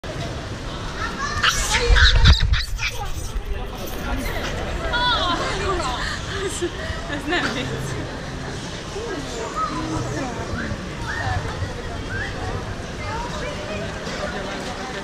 Monkey fight | GoPro Travel Diary: Malaysia
We went to visit one of the most popular Hindu Shrines outside of India. Unfortunately you cannot really enjoy the spiritual environment - there are a lot of people who feed the monkeys with all kind of junkie food, even it's prohibited. It looks more like a crowded zoo with all kind of monkeys instead of a spiritual place.

GoPro video: GoPro 5 Black
Location: Batu Caves, Murugan Temple - Malaysia